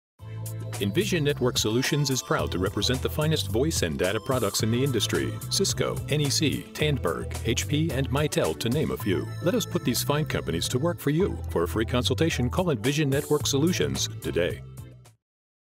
Envision Networked Solutions -- Honolulu, HI
Envision Networked Solutions is a Hawaii-based provider of converged, voice and data solutions solving problems with comprehensive, integrated solutions to support the business of its clients on all islands. Envision has the size, breadth of solutions, strategic partnerships and a team of 50 of Hawaii's most experienced voice and data professionals to support end-to-end communications needs.